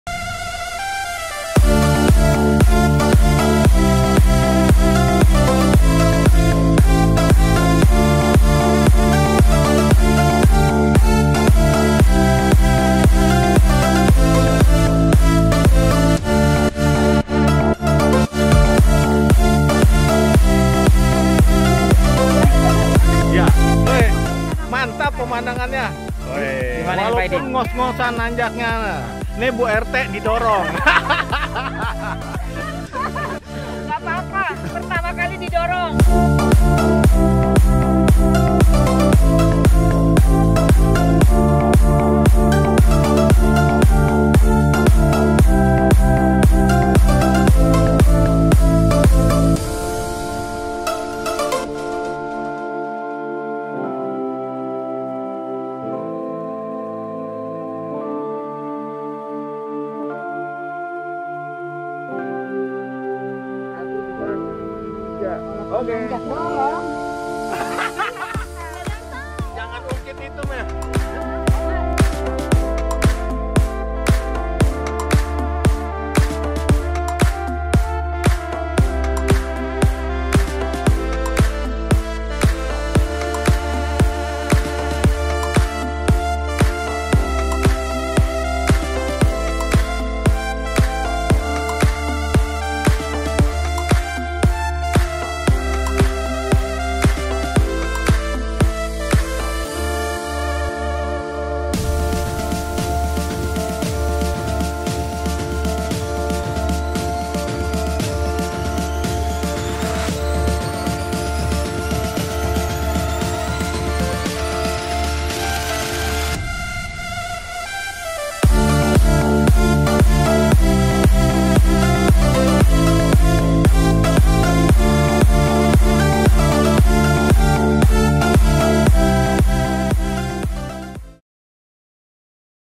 taman budaya sentul tempat aman buat latihan sepeda di bogor, gowes di taman budaya sentul
taman budaya sentul tempat aman buat latihan sepeda di bogor
engan berbagai tanjakan sedang dan berat, baik yang profeional maupun amatir cocok latihan disini
terimakaih BA cycling